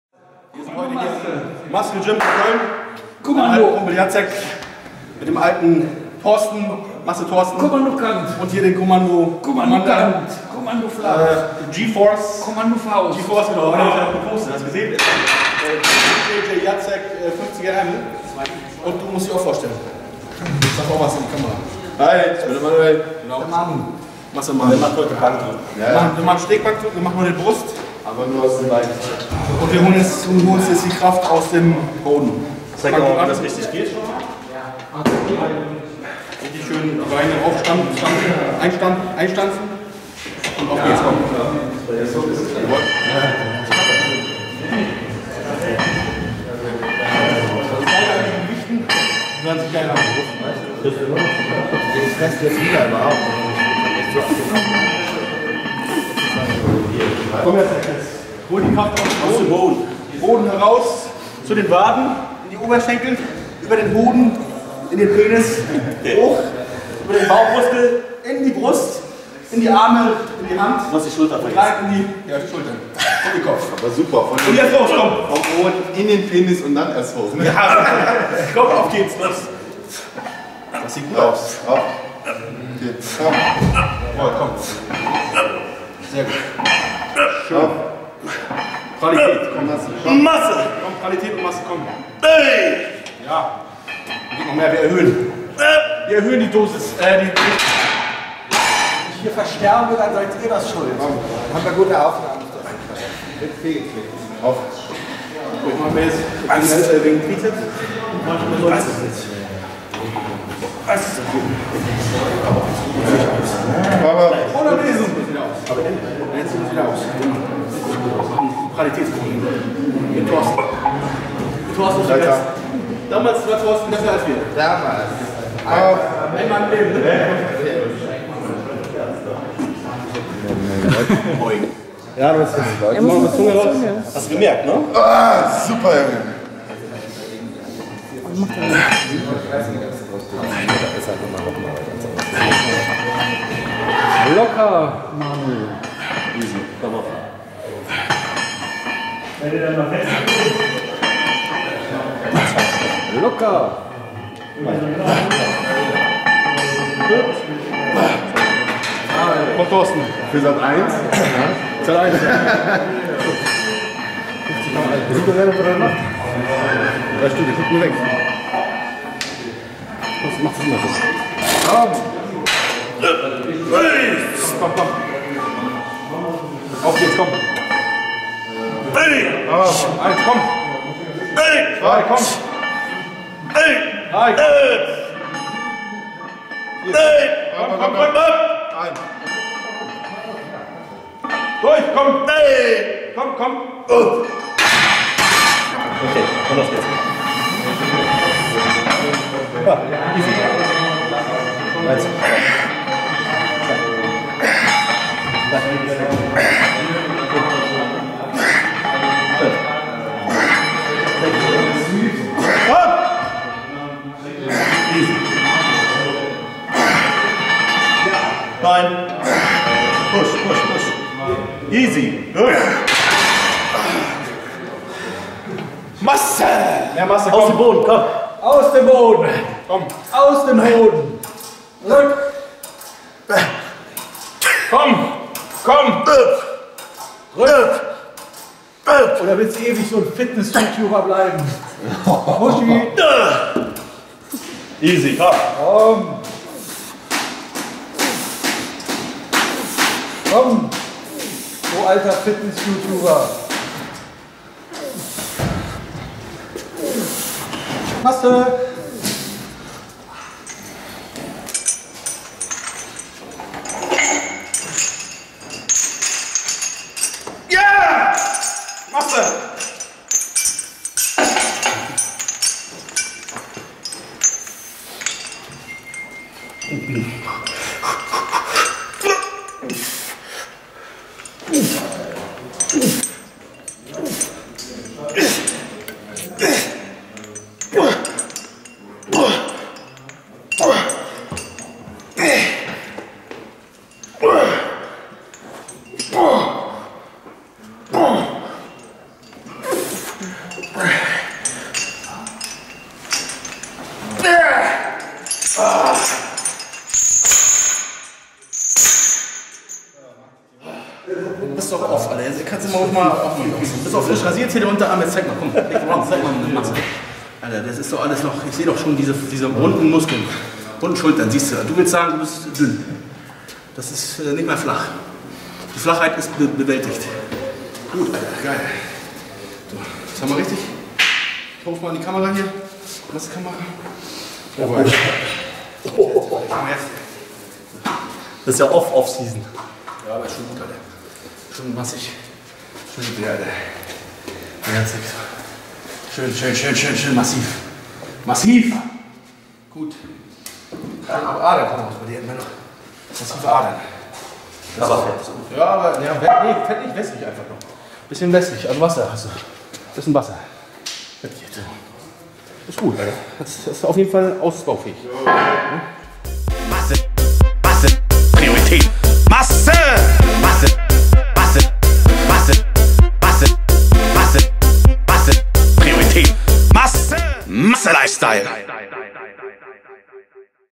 Bankdrücken mit der Kraft aus dem Boden
Dieser Tage war ich in NRW unterwegs und es ging ins Muscle Gym Cologne mit meinen alten Freunden zum Brust ballern